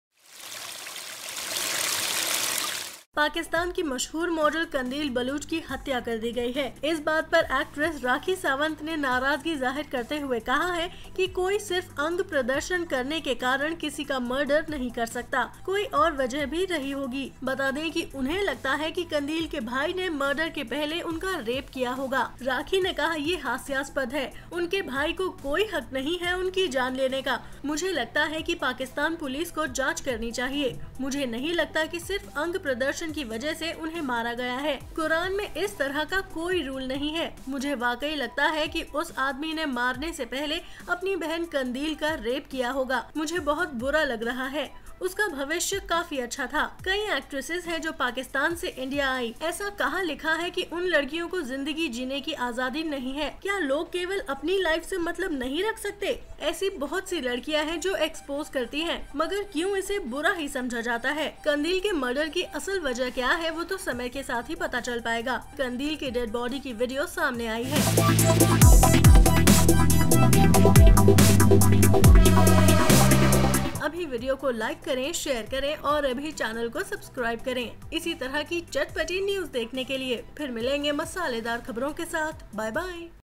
Rakhi Sawant Claims Qandeel Baloch Molested By Her Brother
Rakhi Sawant  Claims Qandeel Baloch Raped By Her Brother